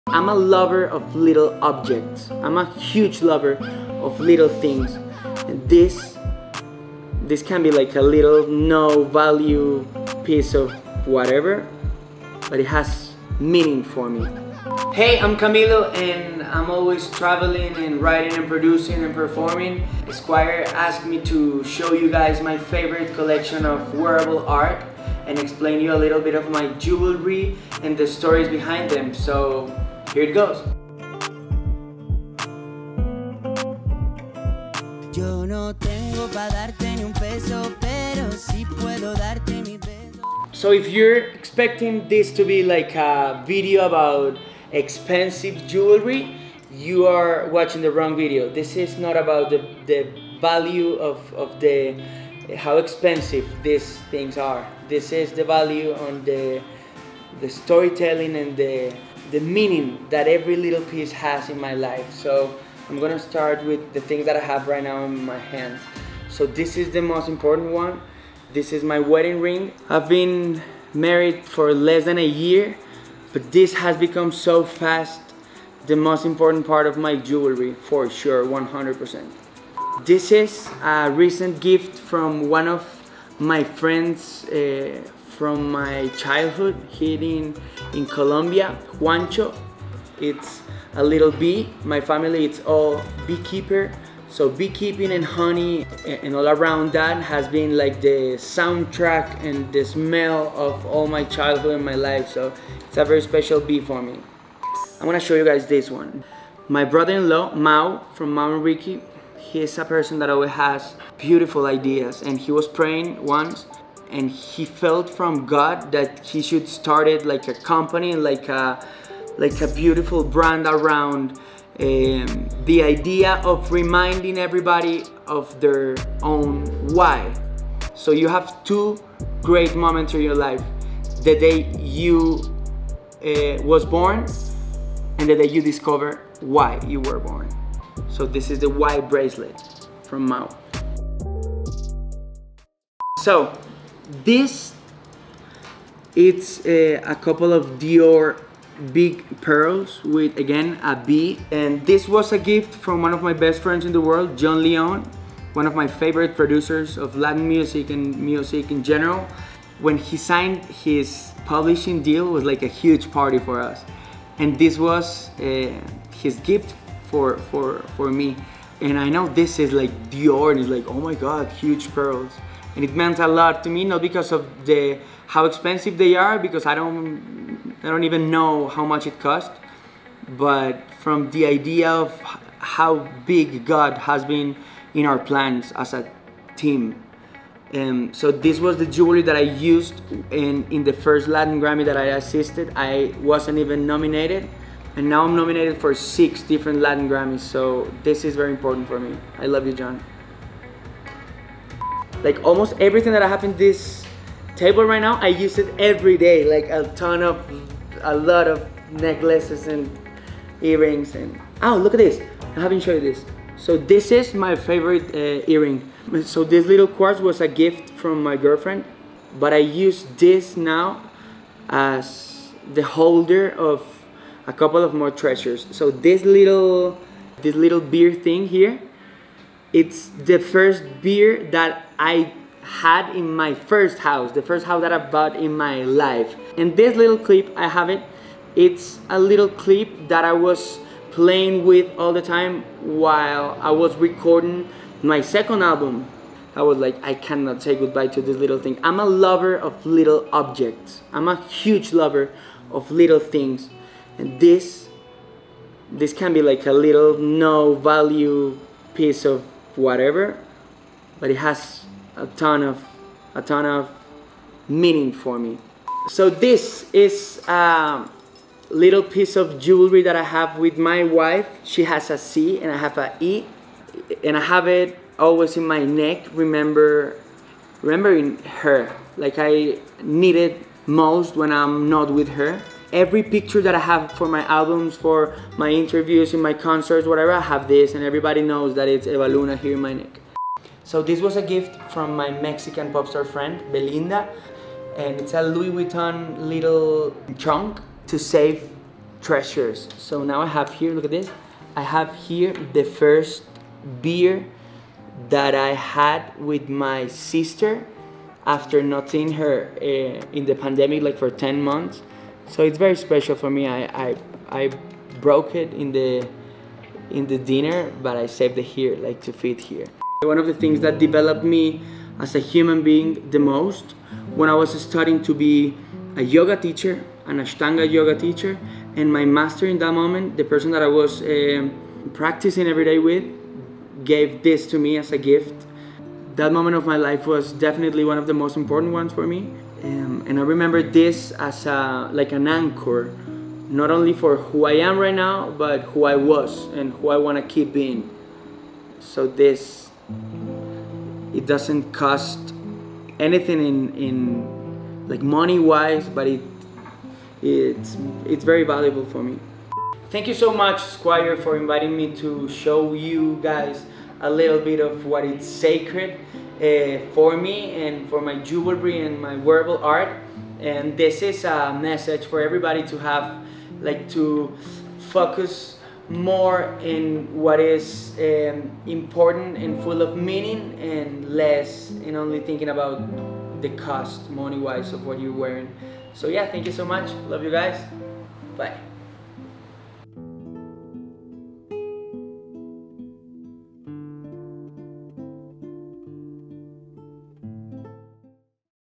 Camilo Opens Up His Jewelry Collection and Home | Curated | Esquire
Camilo shows his sentimental jewelry collection—from his wedding ring, to a special bracelet, to Dior pearls—and tells us the stories of each piece.